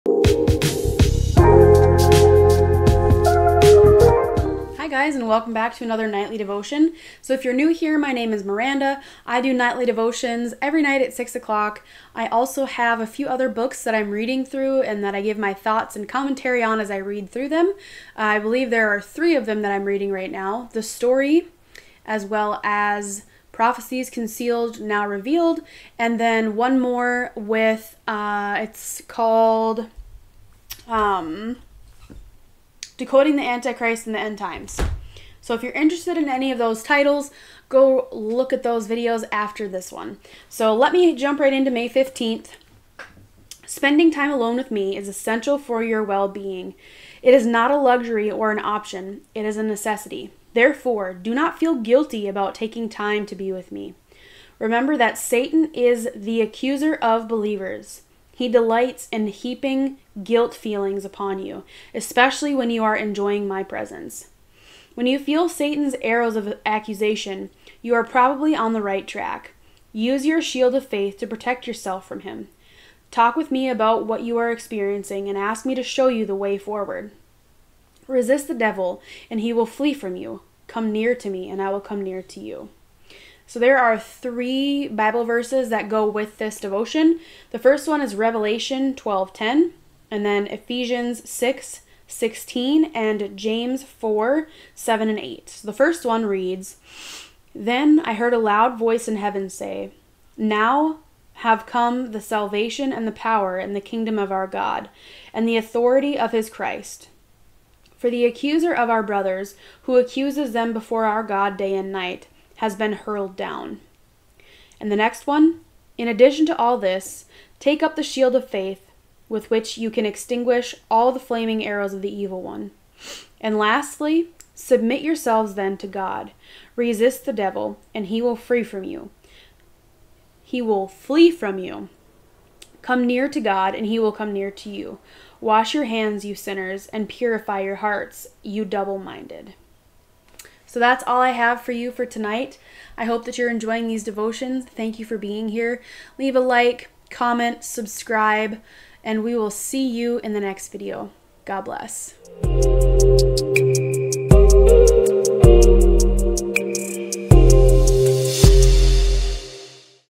May 15th
Enjoy my nightly devotions read out of Jesus Calling by Sarah Young!